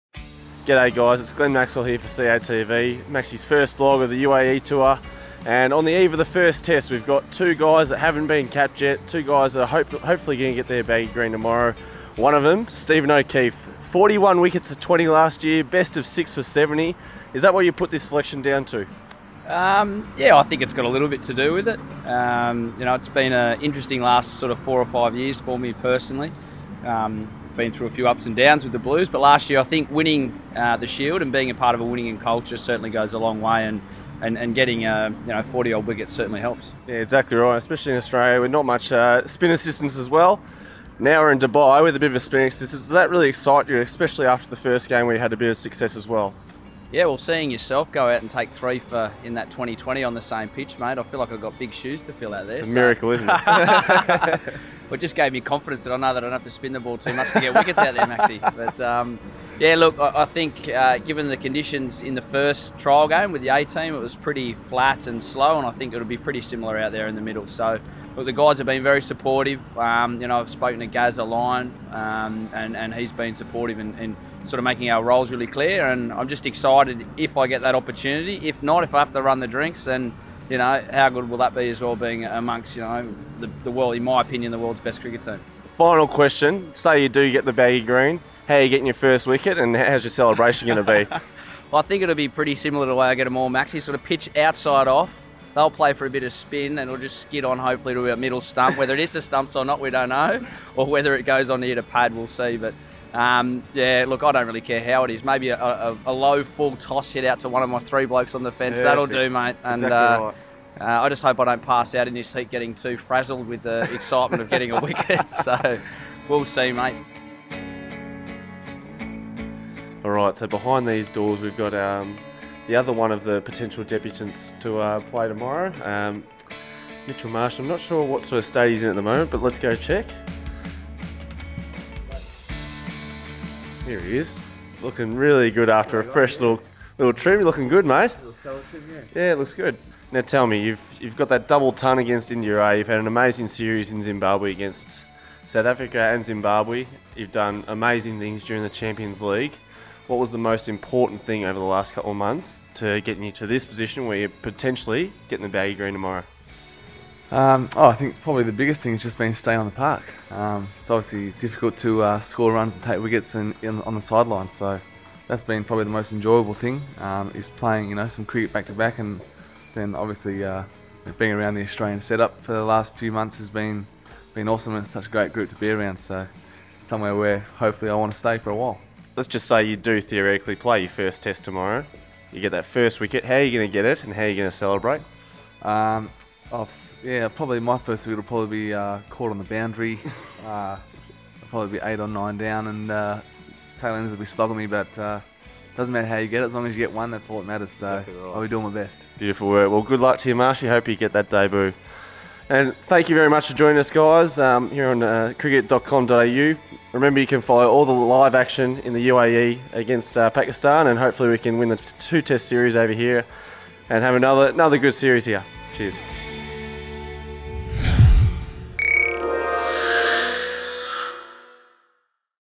Maxi and the new men
Aussie allrounder Glenn Maxwell speaks with Mitch Marsh and Steve O'Keefe ahead of the first Test against Pakistan.